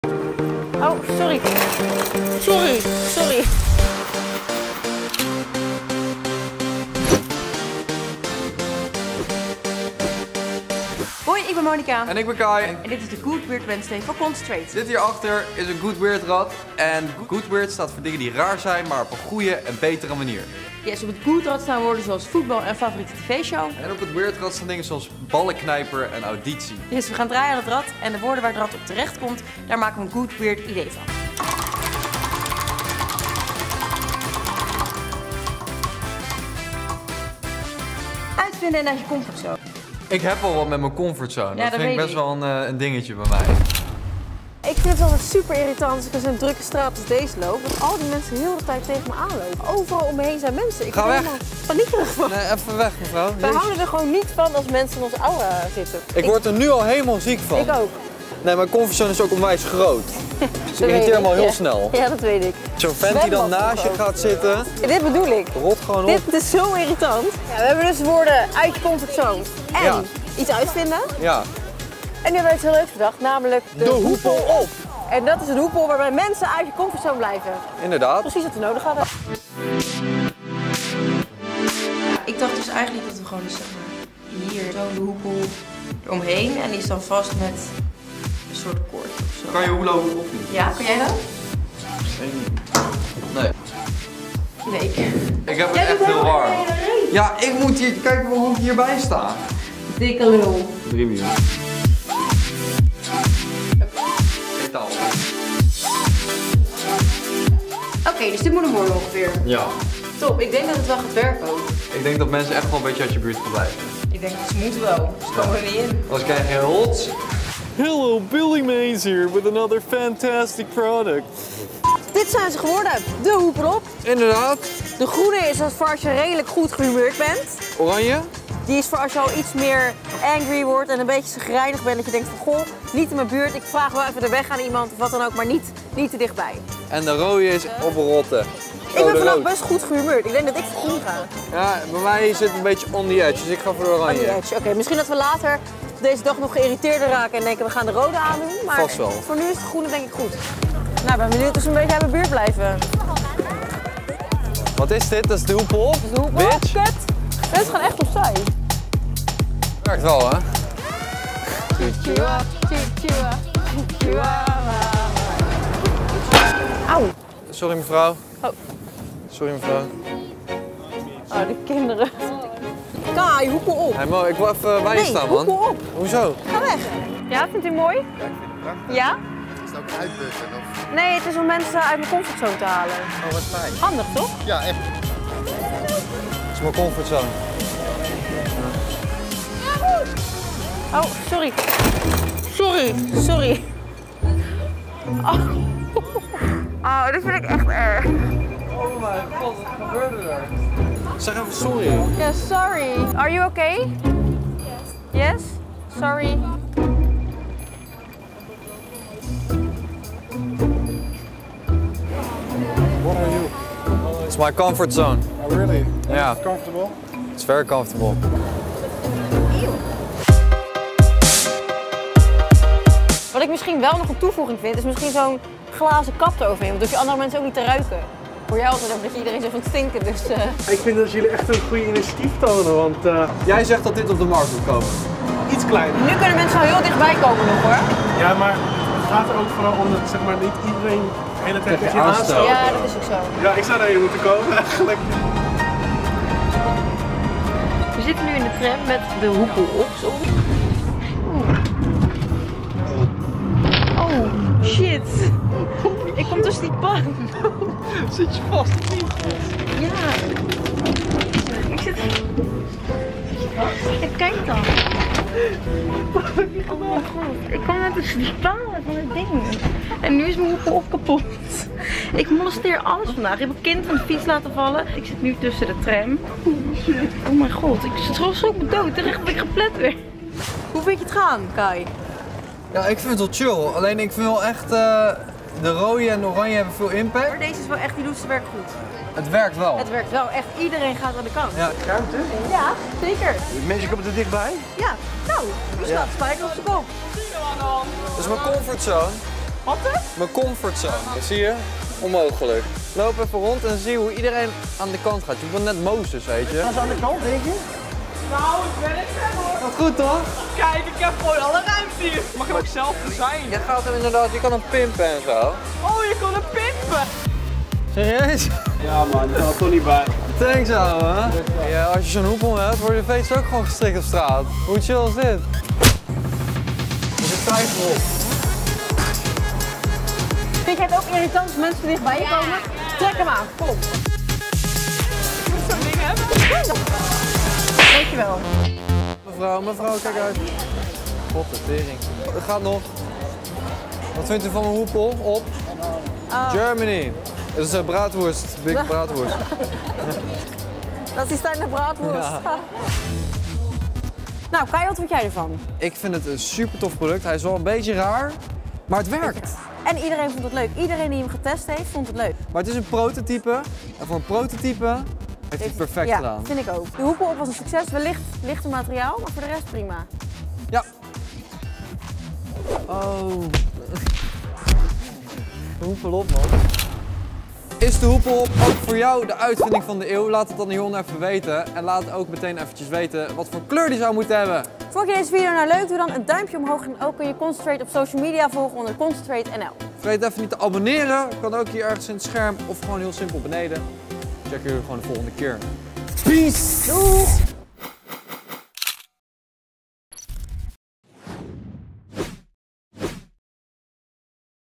ROT OP UIT MIJN AURA! - CONCENTRATE E
Deze video wordt mede mogelijk gemaakt door Lenovo.

We starten weer met een nieuwe reeks Goodweirdwednesday. Mo en Kaj houden mensen met de 'Hoepel Op' uit hun comfortzone.